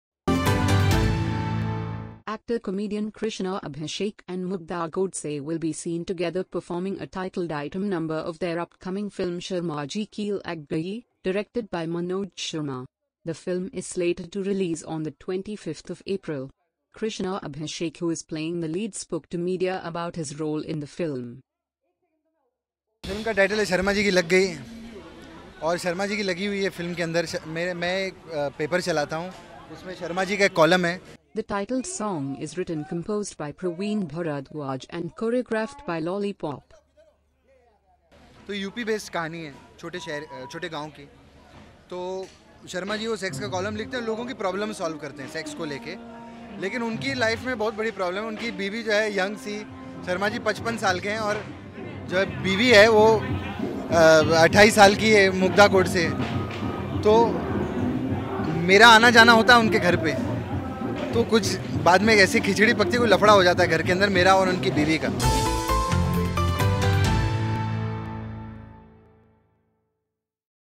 Krushna Abhishek & Mugdha Godse Shoot For An Item Number
Actor-comedian Krushna Abhishek and Mugdha Godse will be seen together performing a titled item number of their upcoming film ‘Sharma Ji Ki Lag Gayi’, directed by Manoj Sharma. The film is slated to release on 25 April. Krushna Abhishek who is playing the lead spoke to media about his role in the film.